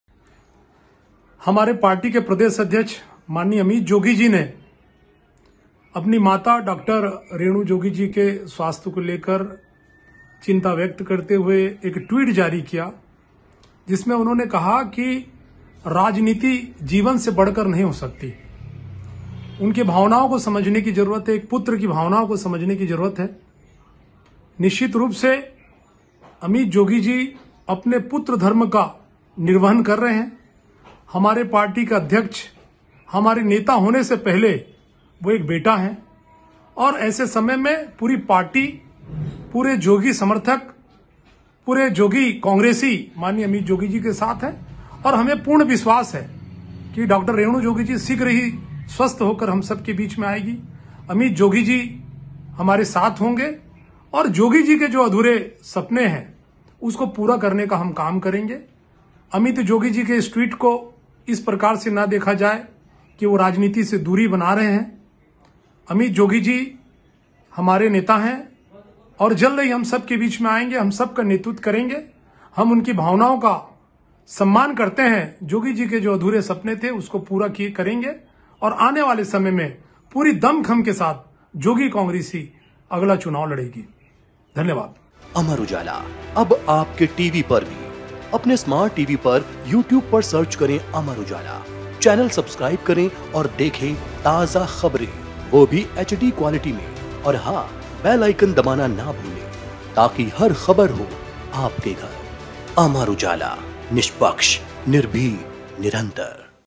Raipur News: छत्तीसगढ़ विधानसभा चुनाव नहीं लड़ेगा जोगी परिवार ! | Chhattisgarh News
छत्तीसगढ़ में इस साल विधानसभा चुनाव होने हैं। इस बीच राजनीति के गलियारे से बड़ी खबर सामने आ रही है। इस बार के विधानसभा चुनाव से जोगी परिवार खुद को दूर रख सकता है। इसकी जानकारी खुद जेसीसीजे के प्रदेश अध्यक्ष अमित जोगी ने दी है। अमित जोगी ने इस संबंध में सोशल मीडिया पर पोस्ट कर अपनी बात लोगों के सामने रखी है।